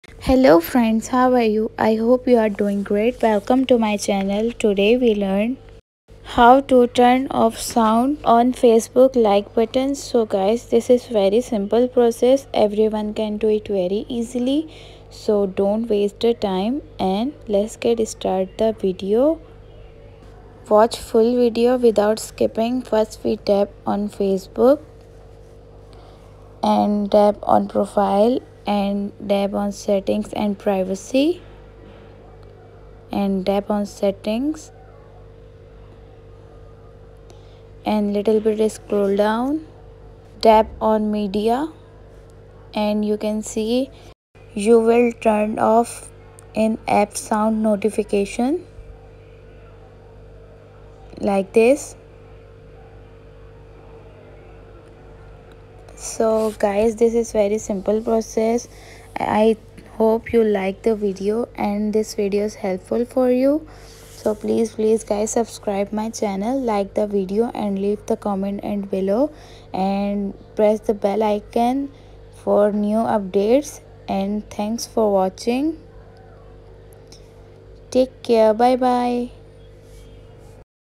How to Turn Off Sound on Facebook Like Button (NEW UPDATE in 2023)
In this video I'll show you How to Disable the Facebook Like Button's Sound (NEW UPDATE in 2023). The method is very simple and clearly described in the video. Follow all of the steps & How to Turn Off the Sound of the Facebook Like Button (NEW UPDATE in 2023)

Congizant of Tutorial